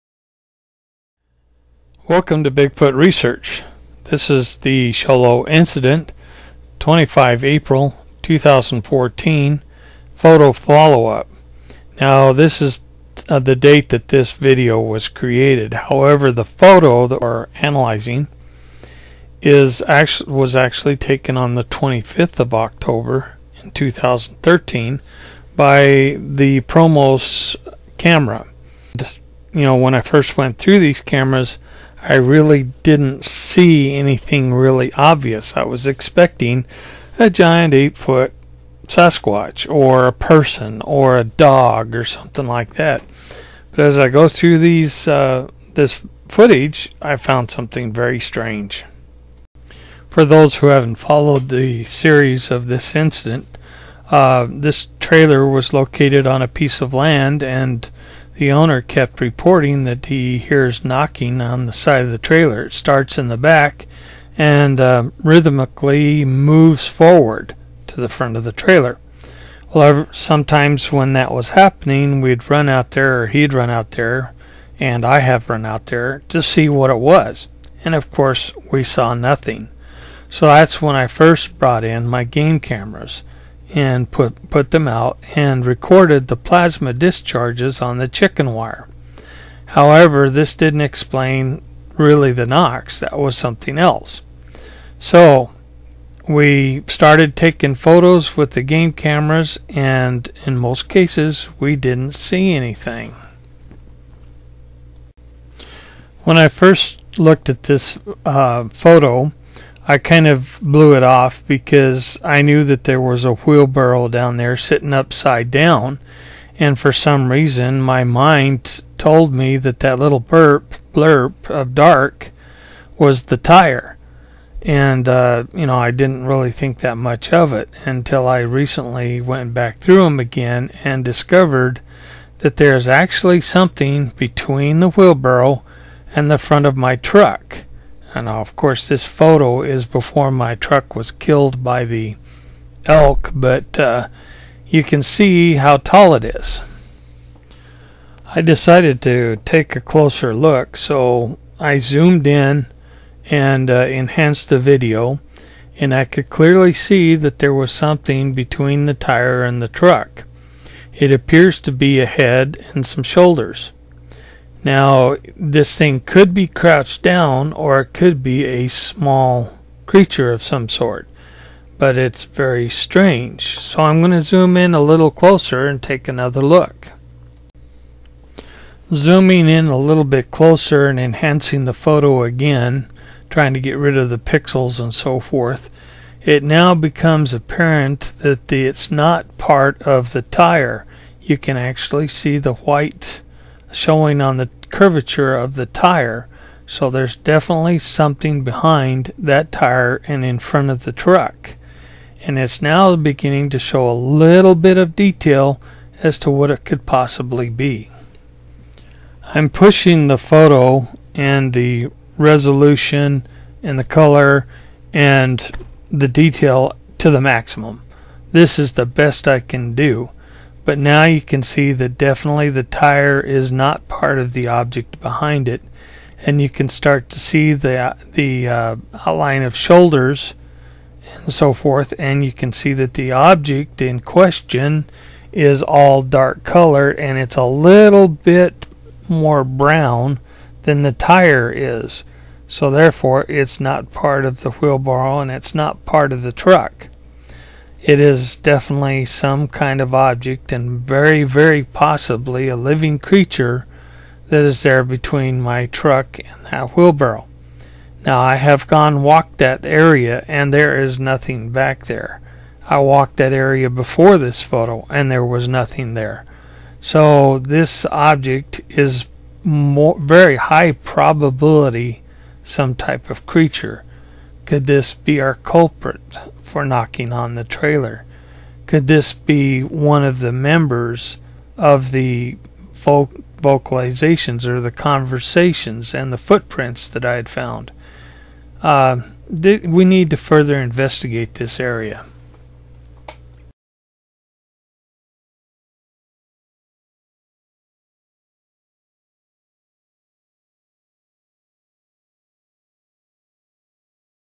BIGFOOT RESEACH SHOW LOW TRAILER INCIDENT PHOTO FOLLOW UP
This photo was taken during the first investigation back in 25 October, 2014 at the trailer near Show Low.  At first we did not see anything, but upon closer look, there seems to be a figure crouching behind the wheel barrel in front of the truck.  We take a closer look. Who or what is unknown but strange.